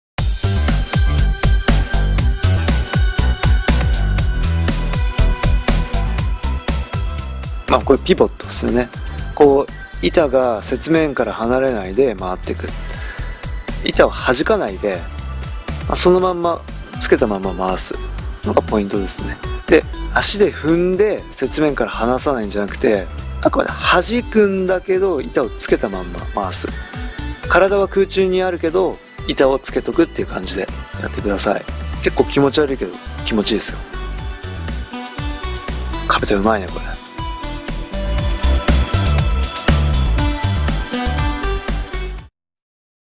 【グラトリHow to】コンパス / ピボット360(解説付) from Technician9【Sclover】
グラトリのコンパス又は、ピボットのHow toです。

【WEBサイト】
▶︎壁田竜一オフィシャルサイト